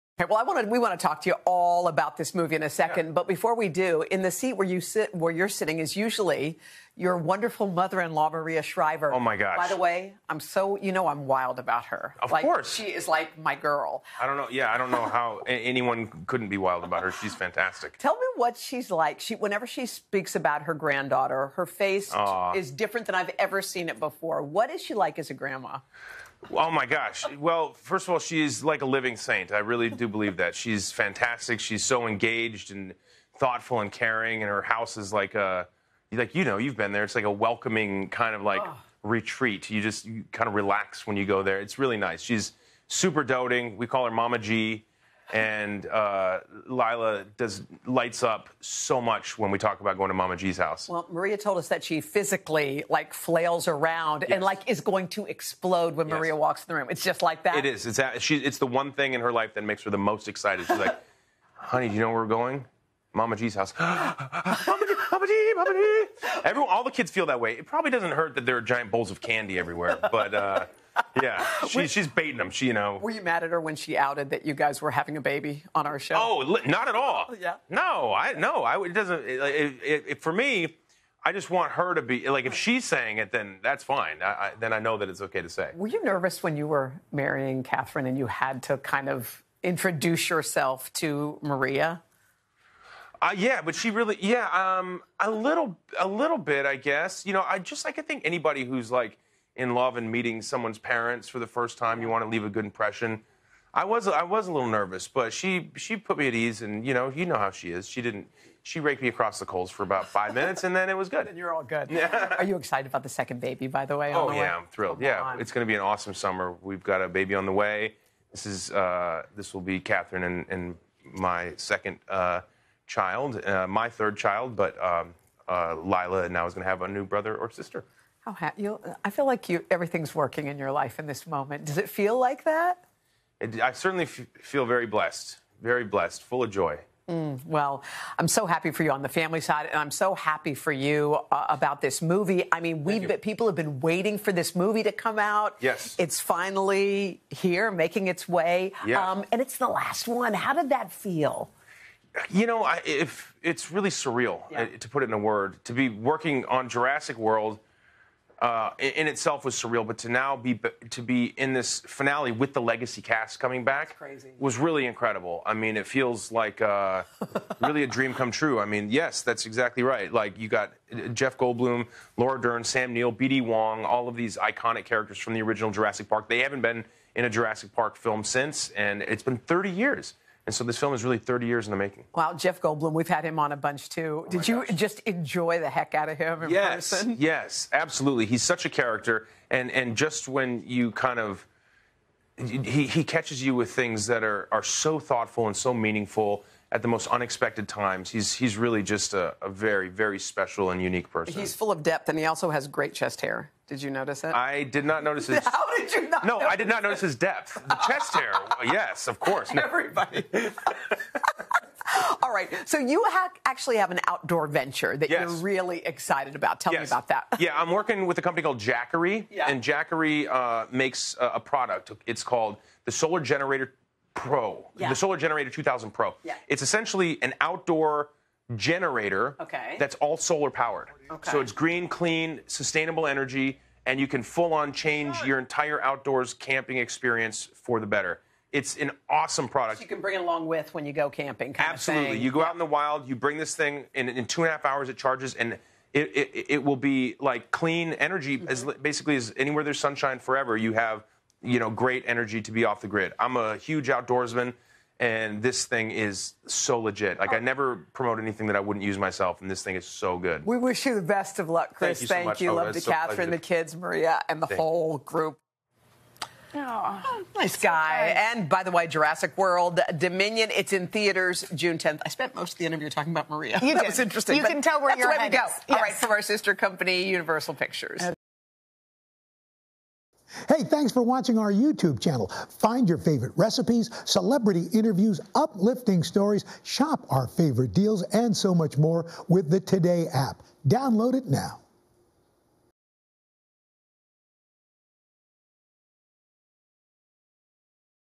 Chris Pratt On What It’s Like Having Maria Shriver As Mother-In-Law
Chris Pratt dishes on the release of “Jurassic World Dominion,” and how surreal it was to work with the legacy cast, including Jeff Goldblum and Laura Dern. He also talks about what it’s like having Maria Shriver as a mother-in-law. “She’s a living saint, I really do believe that.”